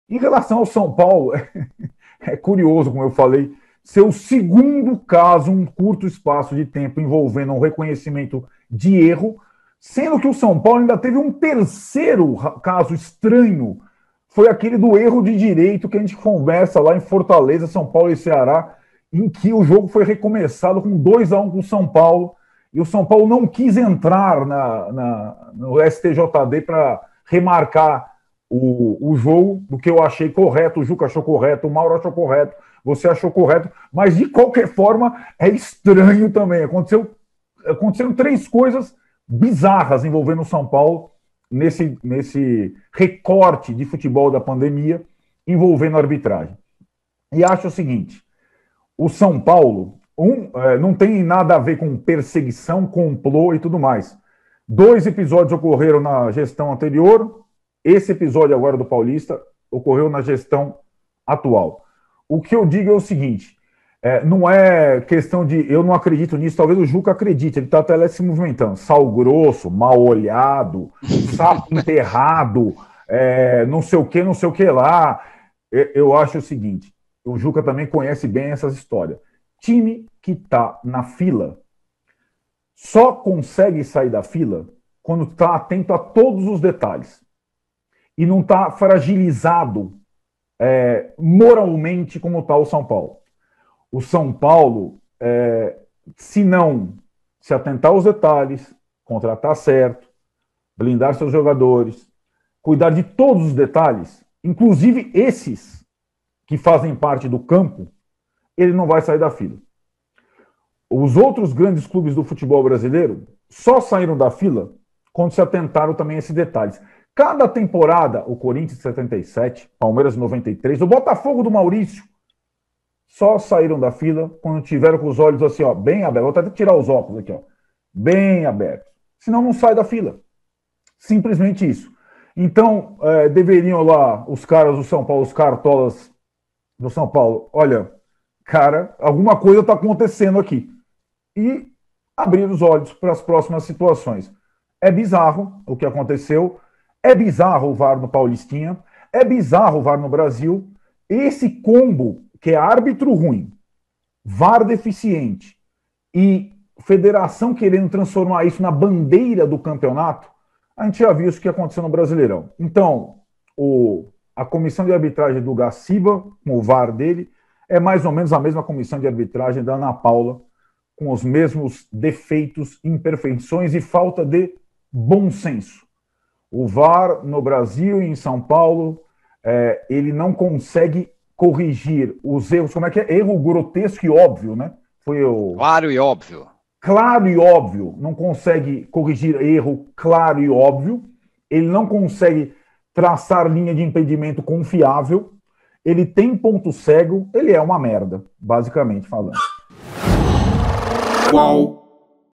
"Time grande só sai da fila quando é atento a todos os detalhes", diz Arnaldo Ribeiro
No episódio #108 do podcast Posse de Bola, Arnaldo Ribeiro afirma que a direção do São Paulo precisa estar mais atenta a todos os detalhes de bastidores para encerrar o jejum de títulos, após série de erros com o uso do VAR no Brasileirão e no Paulista